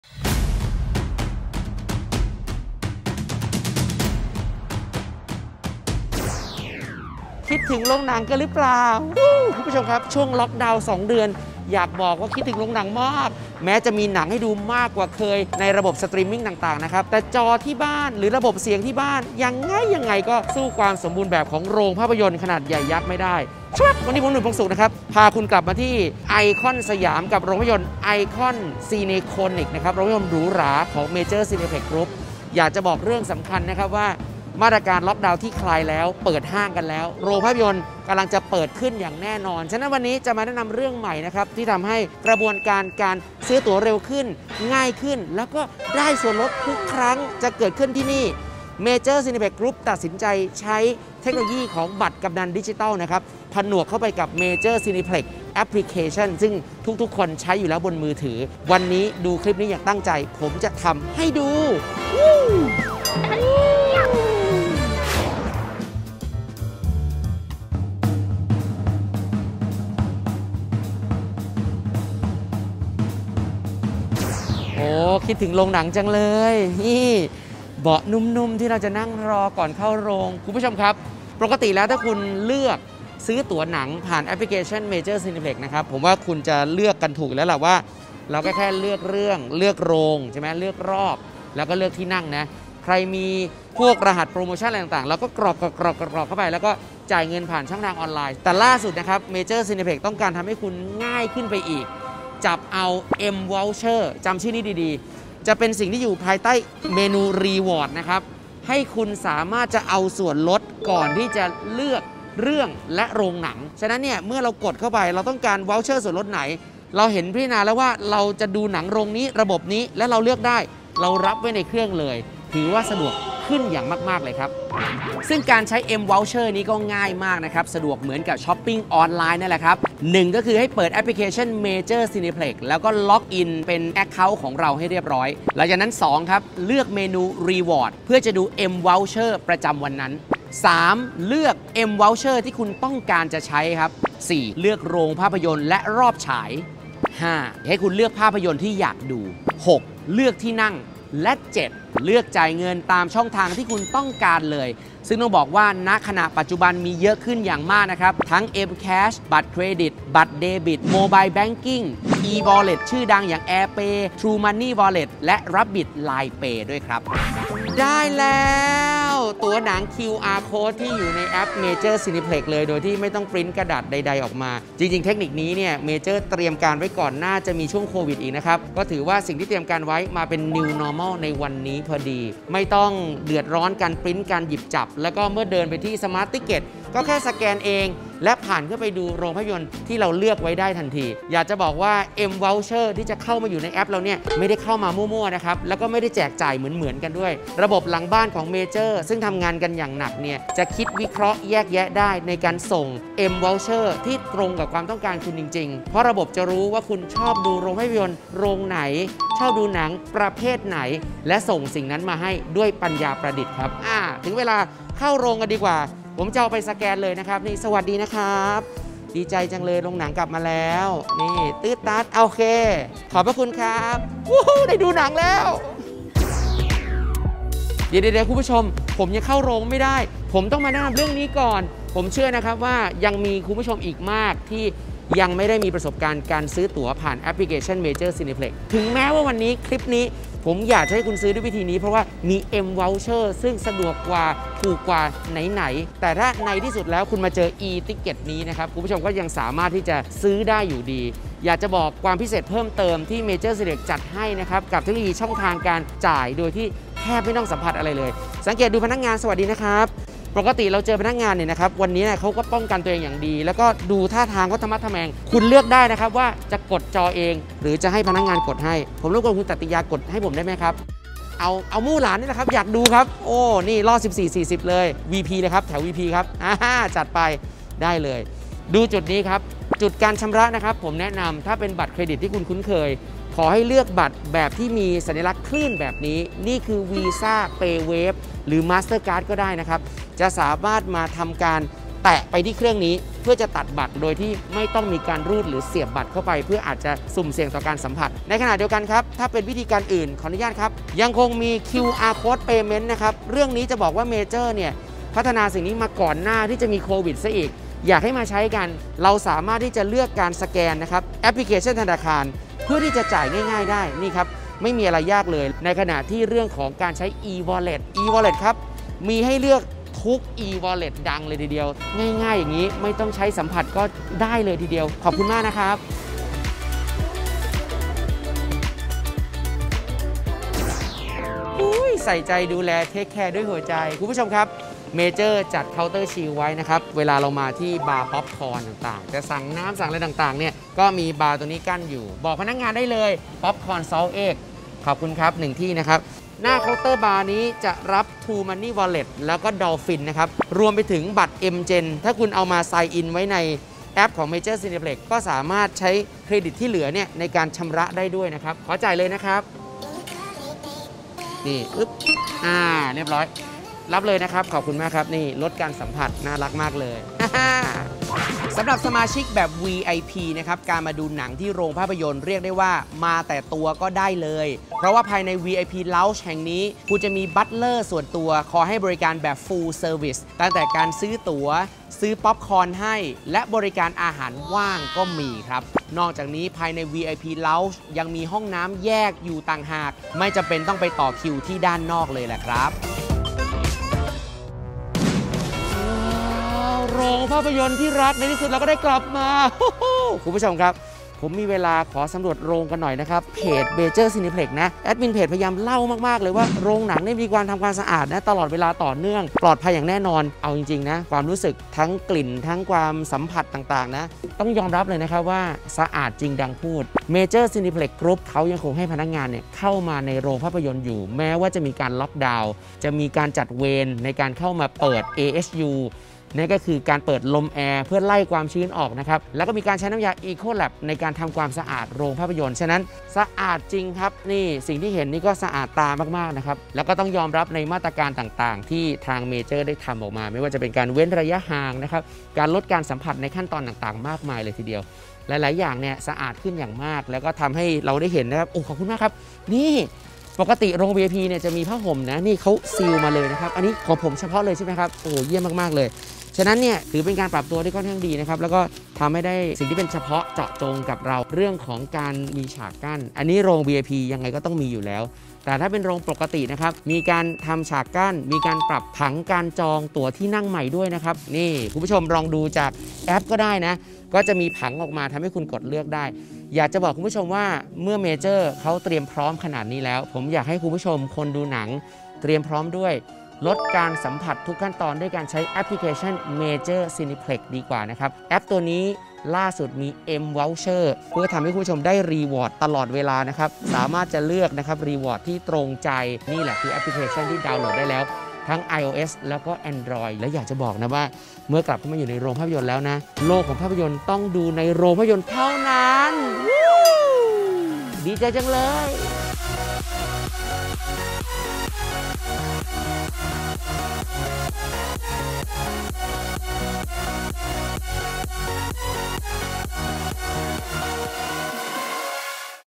Major Cineplex ชวน beartai ดูหนังแบบ Contactless!
การซื้อตั๋วหนังผ่านแอป Major Cineplex หลายคนคงรู้วิธีกันแล้ว แต่วันนี้ beartai จะมาบอกวิธีที่ทำให้คุณซื้อตั๋วหนังได้ถูกลงกว่าเดิมด้วย M Voucher !! แล้วทำได้ยังไงดูคลิปนี้ให้จบ!

ดาวน์โหลดแอปพลิเคชัน MajorCineplex ได้แล้ววันนี้